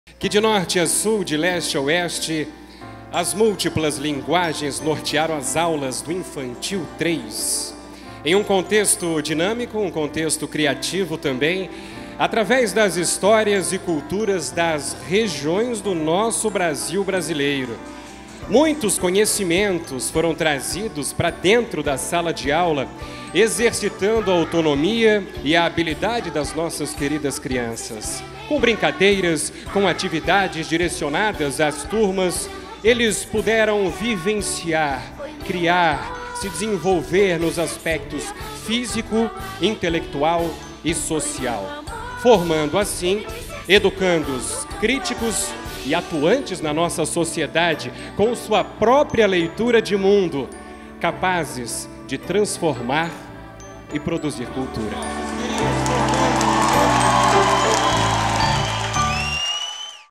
Formatura da Educação Infantil - Colégio Criativo
O Colégio Criativo celebra em grande festa o término da Educação Infantil. No último sábado o Sítio Cataventos recebeu alunos, pais, professores e amigos. Um evento marcado pela construção de muitos conhecimentos, INCRÍVEL! Todos de PARABÉNS!